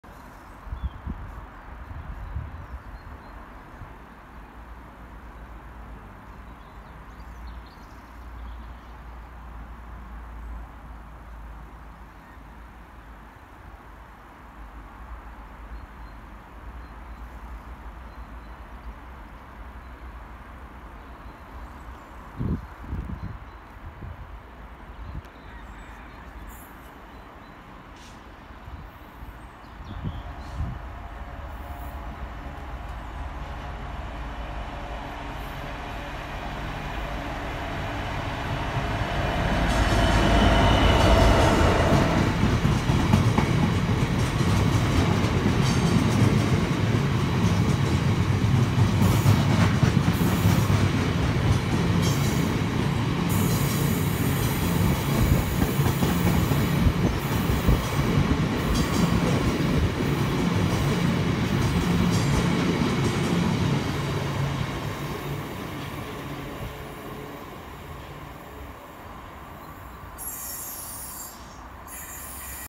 66712 Peterborough power signal box 15 mgr sand wagons 16:57 7L 8/3/2024
6E88 Middleton Towers Gbrf to Goole Glassworks Gbrf 66712 Peterborough power signal box with 15 mgr sand wagons 16:57 running 7 minutes late Lea Road Gainsborough 8/3/2024